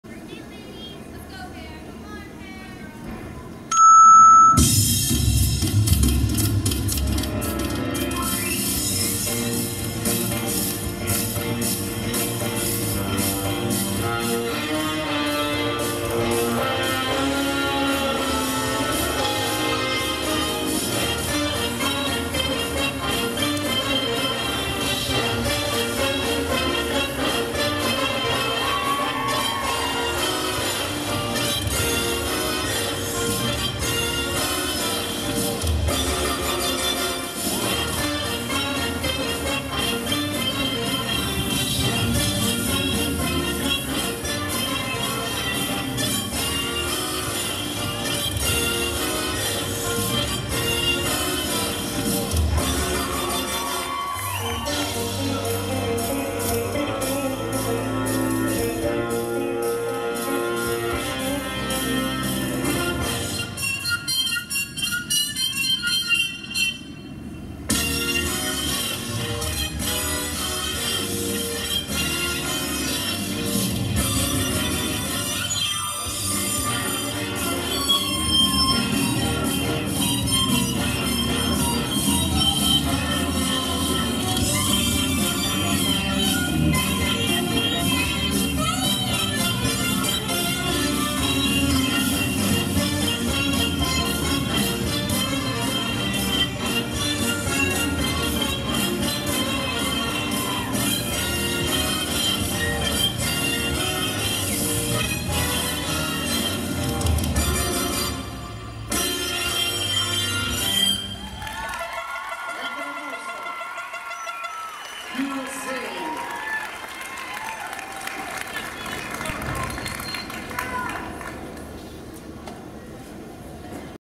PanAm Ch 2015 057 PnA Age 11 16 WP USA2 Dyn DIAMOND & HEINZ
1st PanAmerican Championships 2015
Women's Pair Agegroup 11-16 Dynamic Exercise United States 2,
Sarah DIAMOND & Elizabeth HEINZ